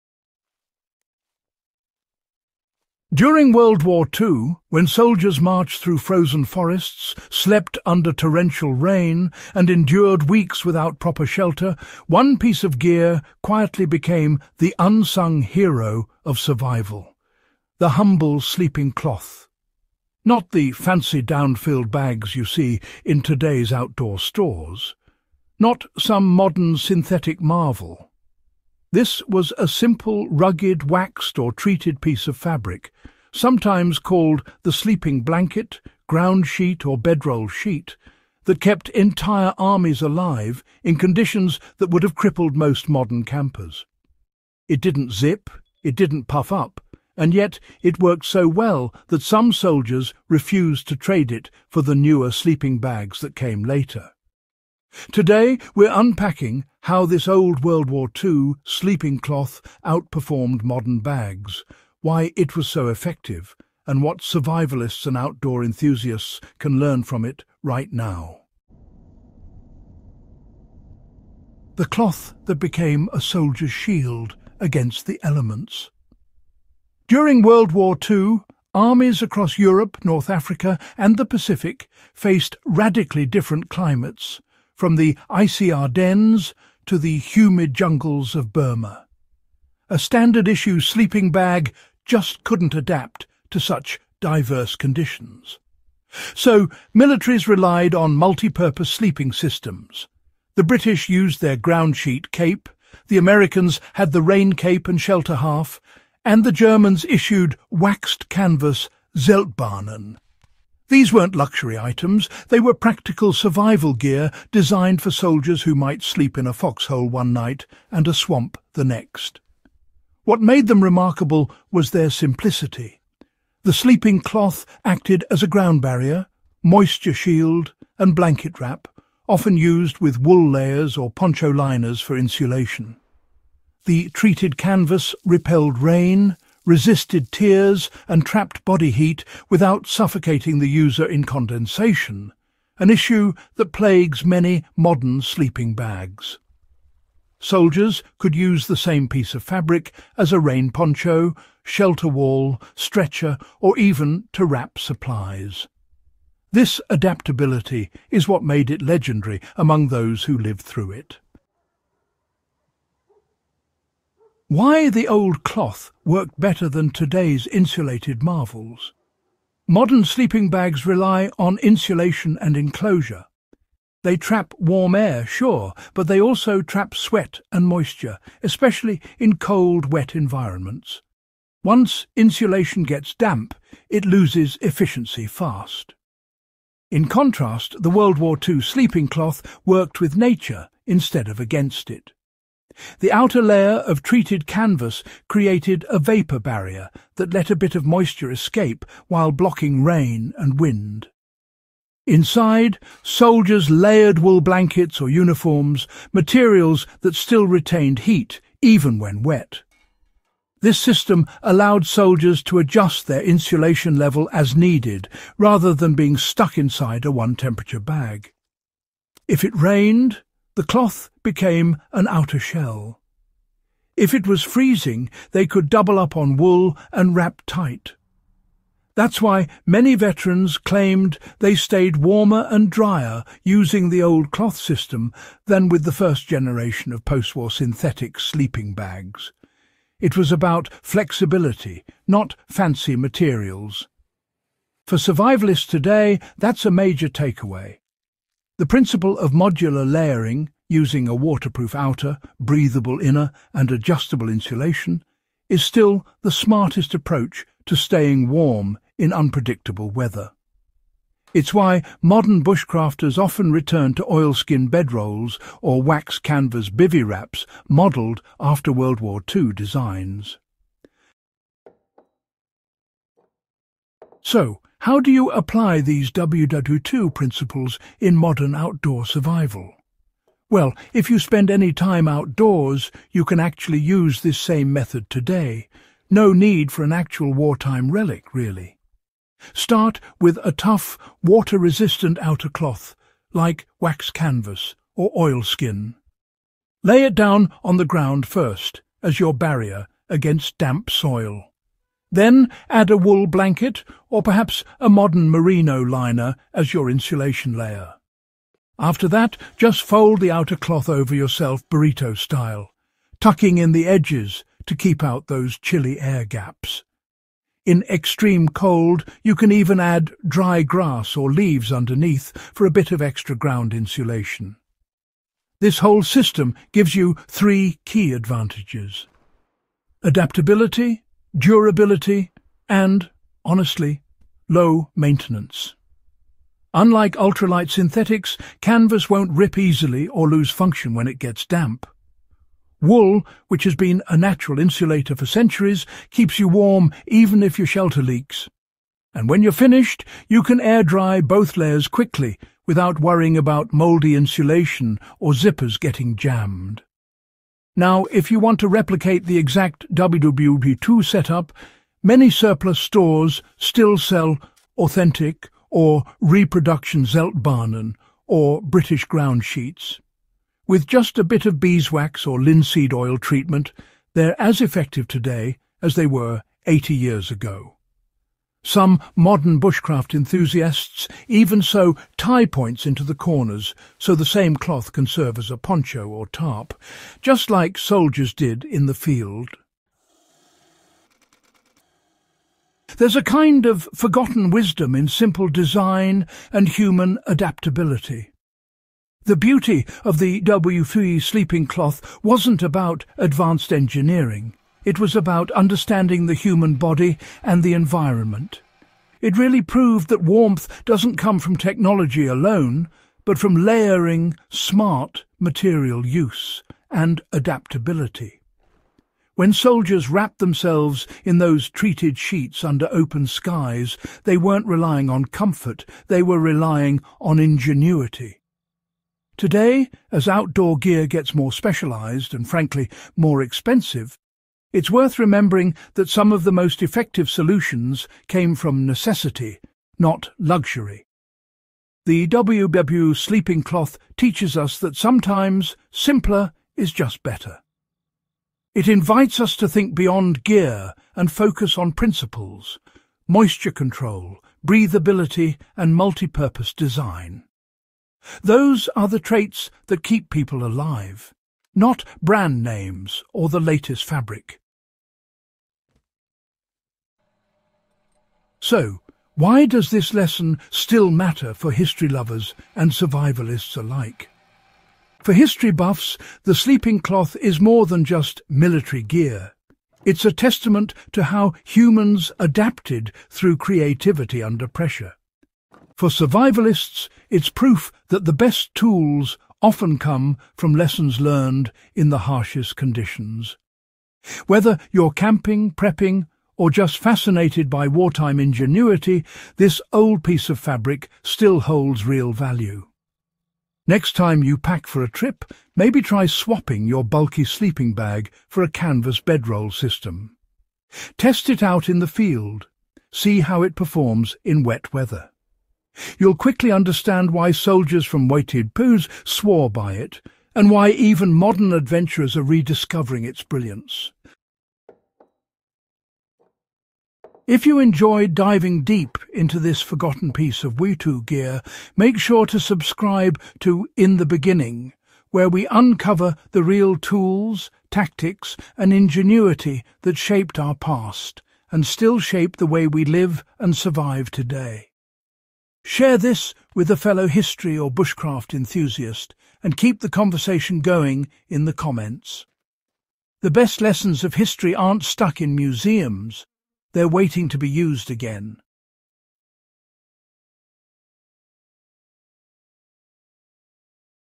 💤 The WWII Sleeping Cloth That Outperformed Modern Bags
Before synthetic sleeping bags and reflective liners, WWII soldiers relied on a simple layered sleeping cloth—built from oil-treated canvas, wool, and ground insulation. It stayed warm when soaked, packed small, and worked in temperatures that froze gear solid. This lost piece of field engineering outperformed modern materials through pure ingenuity. Discover how soldiers built it, why it worked, and how you can replicate its design for cold-weather survival today.

🔥 What You’ll Learn in This Video:

The field-tested layering trick behind the “thermal sandwich” cloth.

How wool and oilcloth combined to trap body heat and repel moisture.

Why this cloth replaced early sleeping bags on multiple fronts.

How to rebuild it safely using modern fire-resistant fabrics.

📌 We Also Answer:

How did soldiers stay warm on frozen ground with no insulation pads?

Can the same method improve modern bivy sacks and tarp setups?